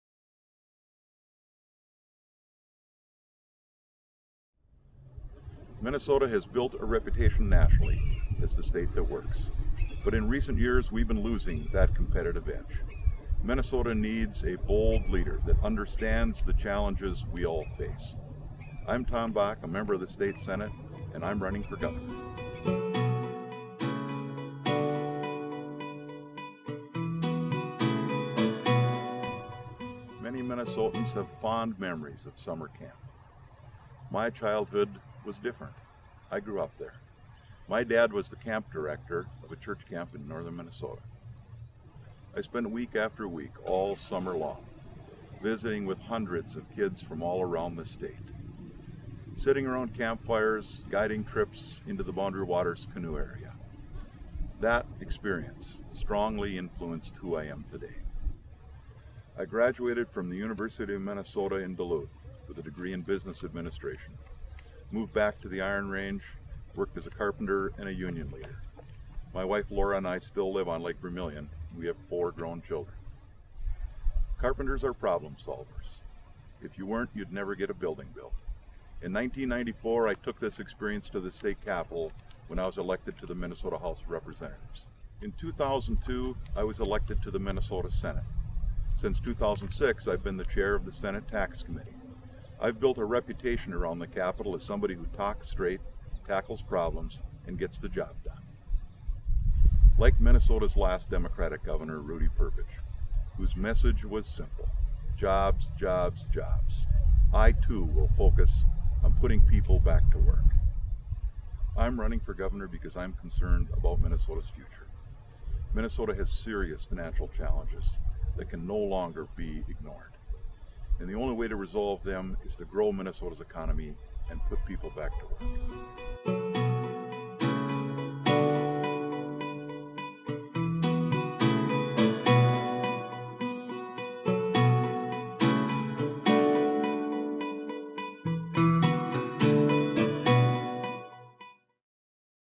Tom Bakk. Real. Results.
Minnesota Senator Tom Bakk is running for Governor.